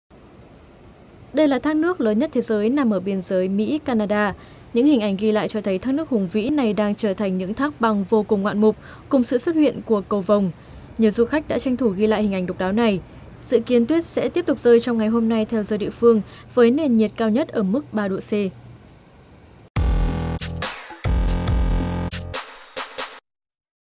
Thác nước Niagara đóng băng thu hút du khách
Nhiệt độ xuống thấp trong nhiều tuần đã khiến ngọn thác Niagara hùng vĩ đóng băng một phần, tạo nên cảnh quan hiếm thấy, thu hút rất nhiều du khách tới chiêm ngưỡng.